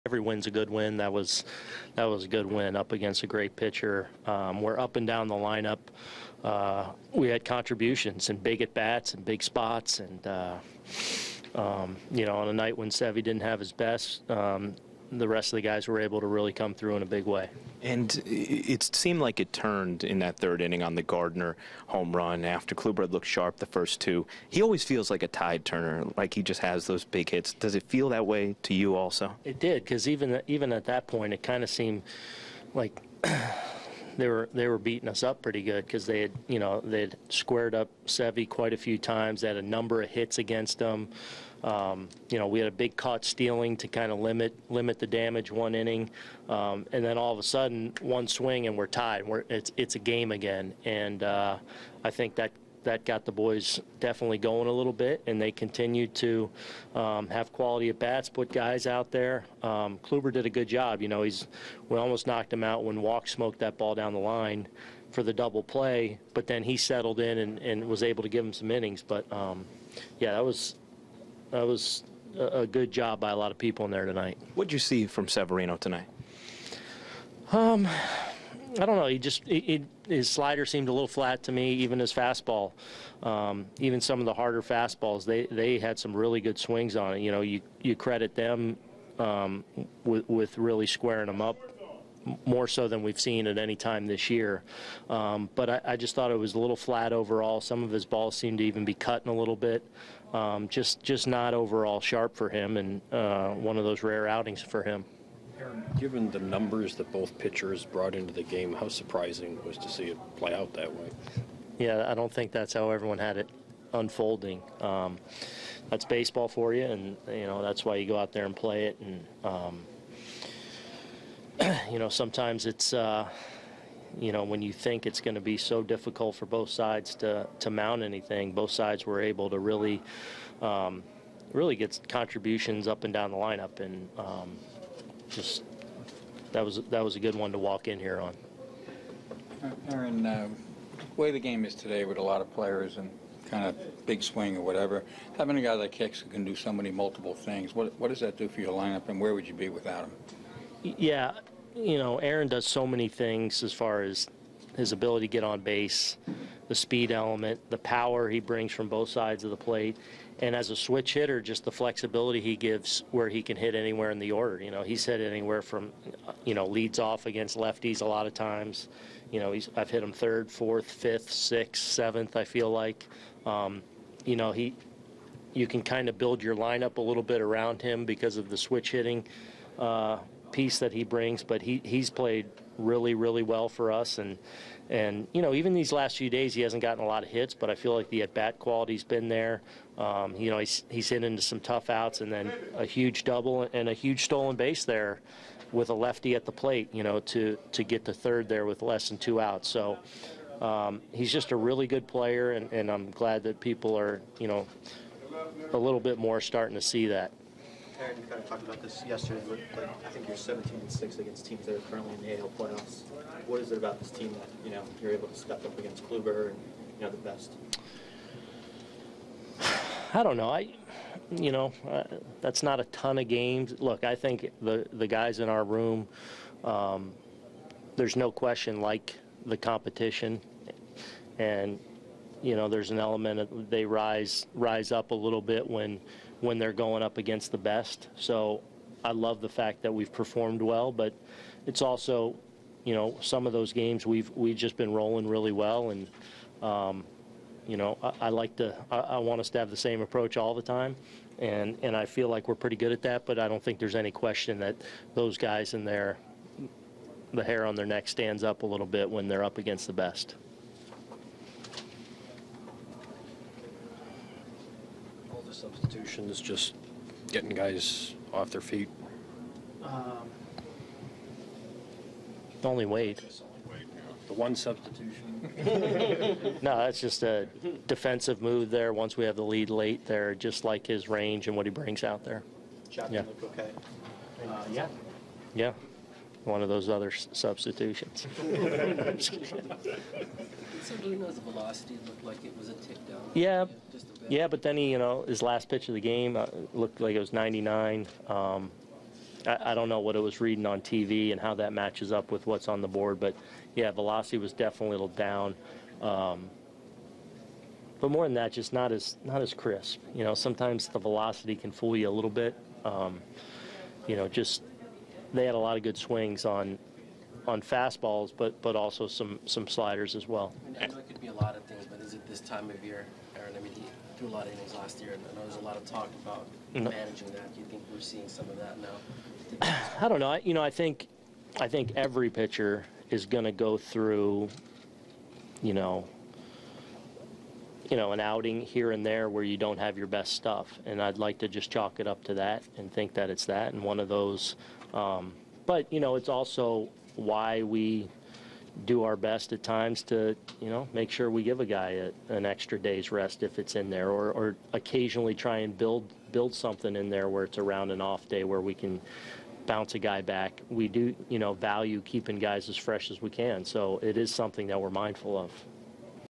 Aaron Boone on the Yankees' Game 1 win in Cleveland
Yankees manager Aaron Boone discusses Luis Severino grinding through five innings and the entire lineup contributing in a 7-4 win over the Indians.